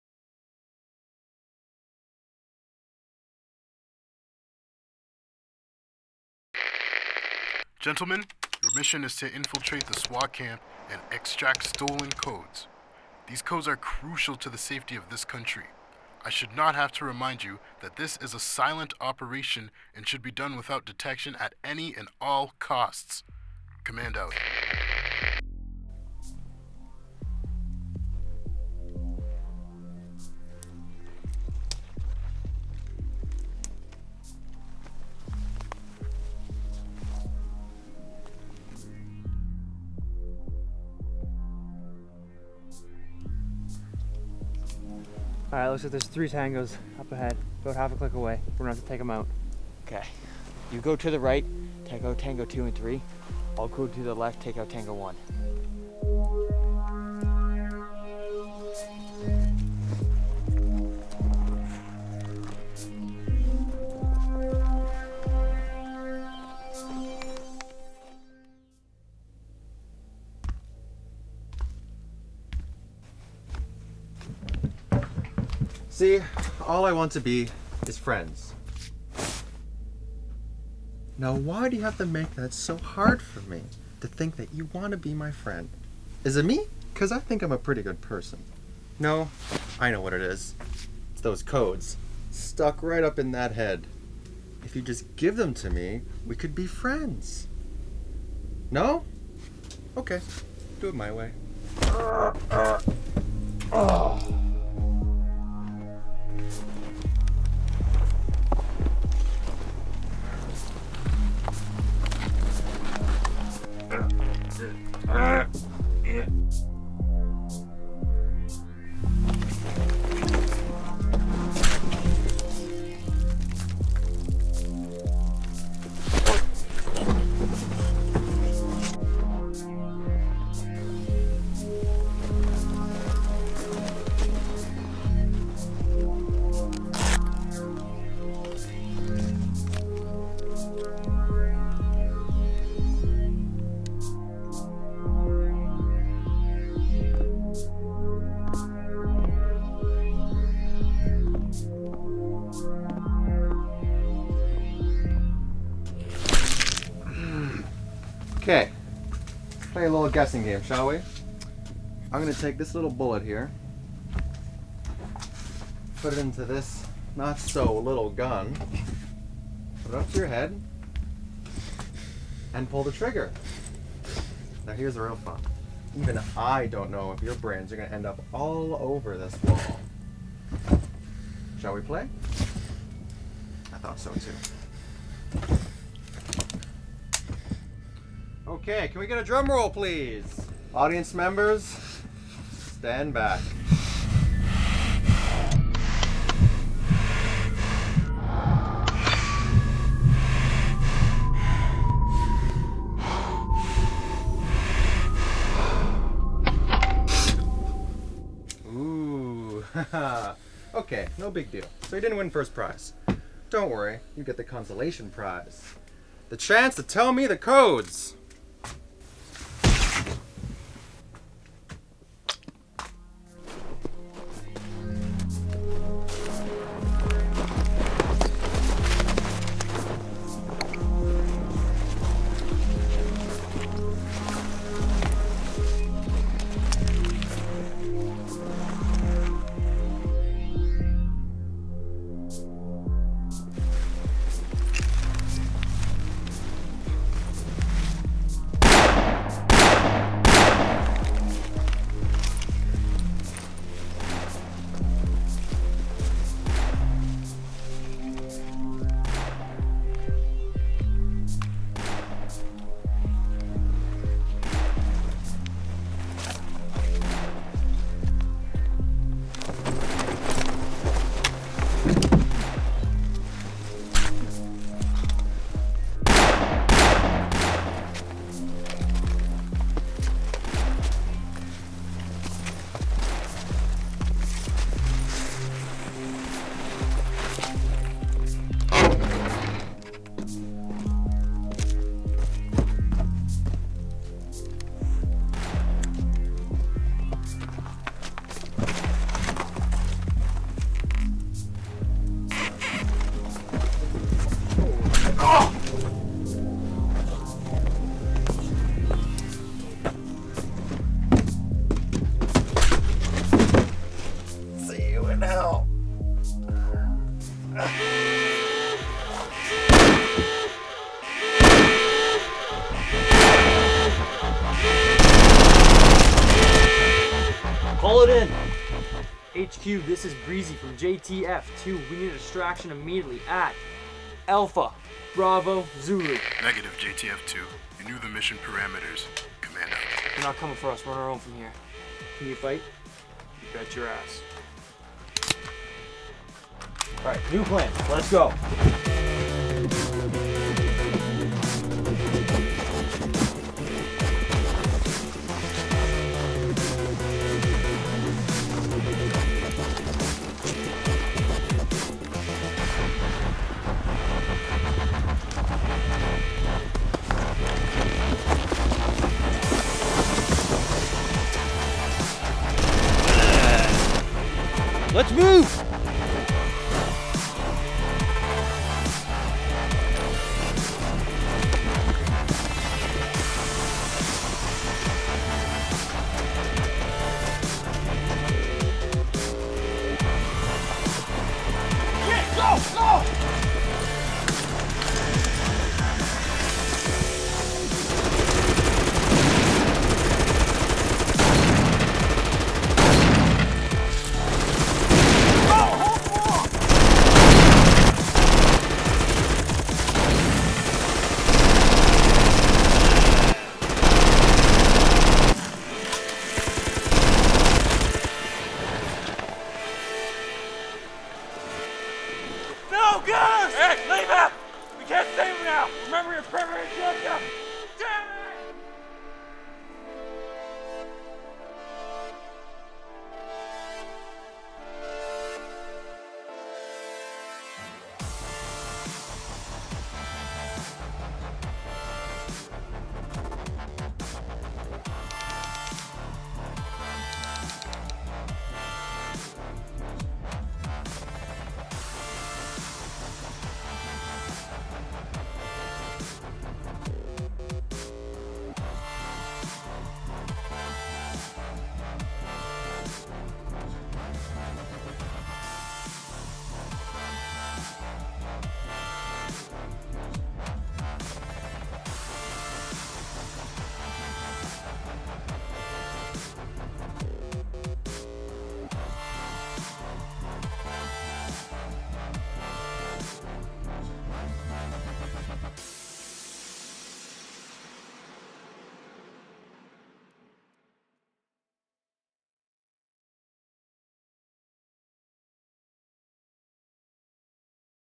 Extraction
This short film is based on the JTF2 (joint task force 2) and there mission to extract stolen codes in a silent operation.